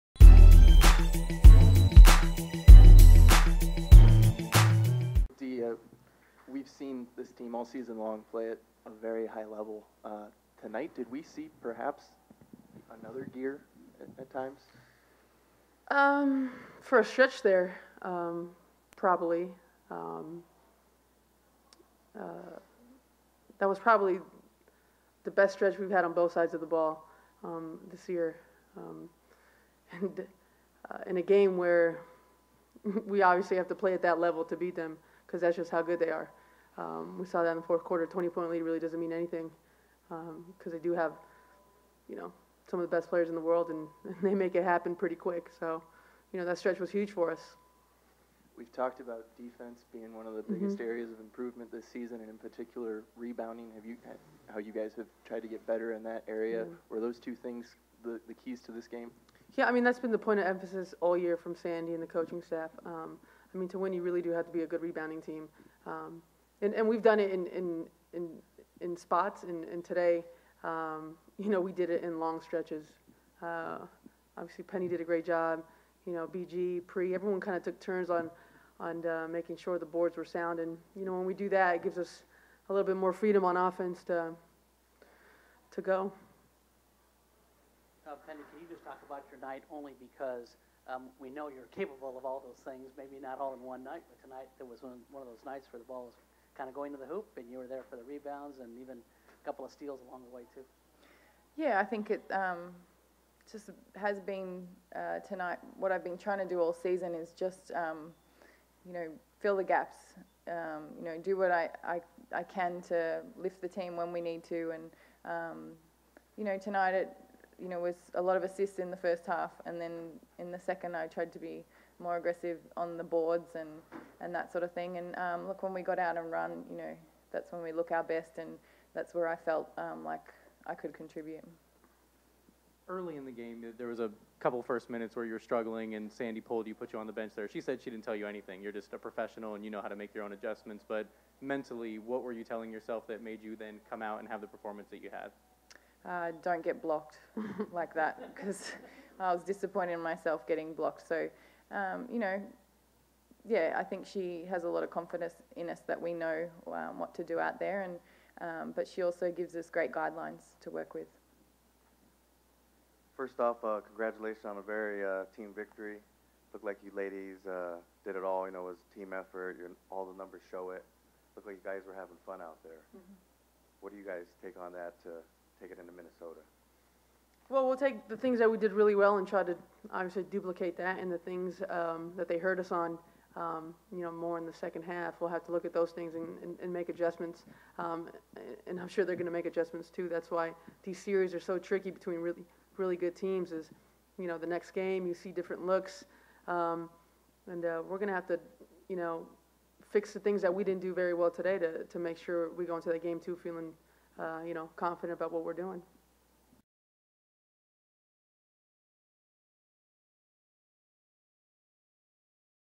Postgame: Taurasi and Taylor - Aug. 29, 2014
Mercury stars Diana Taurasi and Penny Taylor meet with the media to discuss the team's Game 1 win over the Lynx.